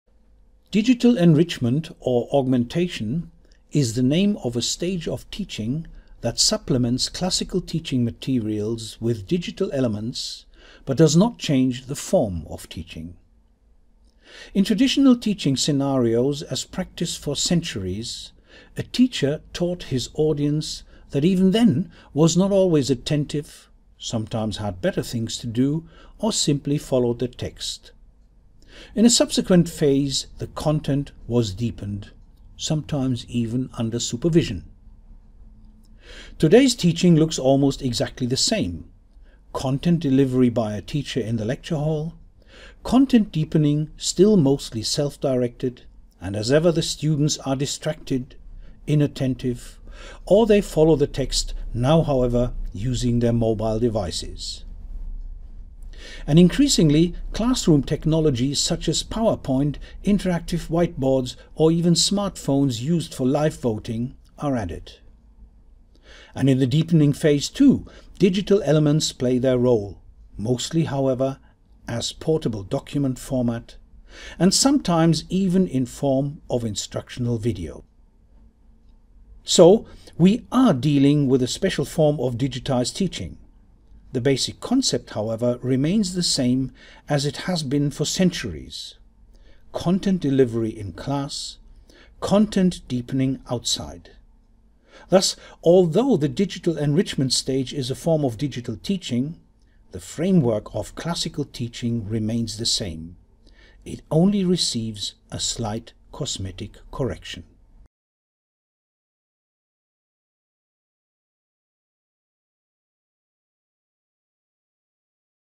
Glossary - Digital Enrichment (Engl.)
What does it mean to enrich or augment teaching and learning digitally? In less than two minutes, Prof. Dr. Handke explains the basic principles of digitally enriched/augmented teaching and learning.